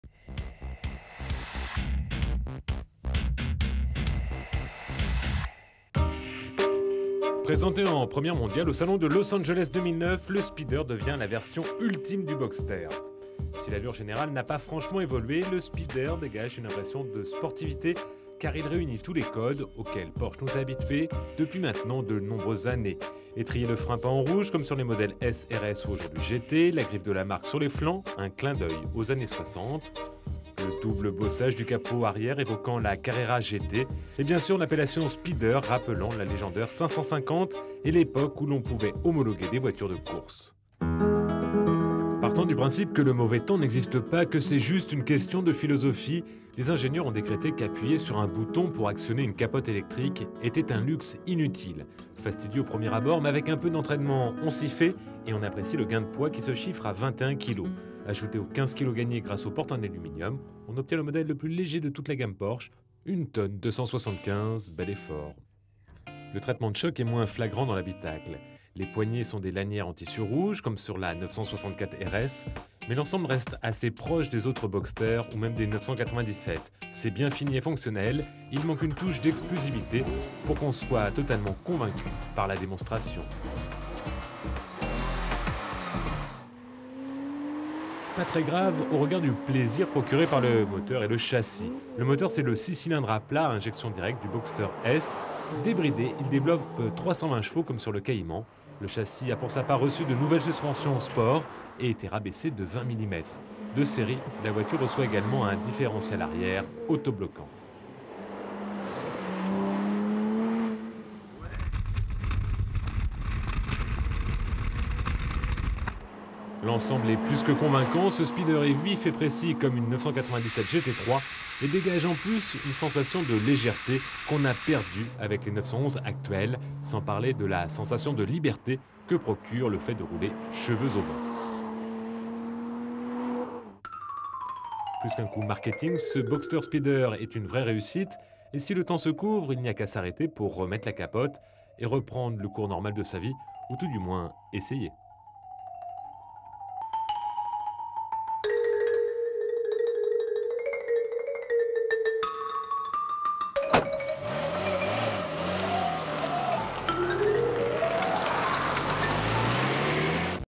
Essai auto PORSCHE BOXSTER SPYDER
Présentée en première mondiale lors du salon automobile de Los Angeles, la version Spyder donne un aspect encore plus sportif au Porsche Boxster, même si les lignes ne sont pas très différentes. Mais le Porsche Boxster Spyder concentre des éléments typiques de la marque, comme des étriers de frein rouges vus sur les modèles S, RS et aujourd'hui GT ; la griffe Porschz apposée sur les flancs comme un clin d'œil aux années 1960. Et, évidemment, la dénomination Spyder, qui ramène la fameuse 550 et le temps où on pouvait homologuer des voitures de course.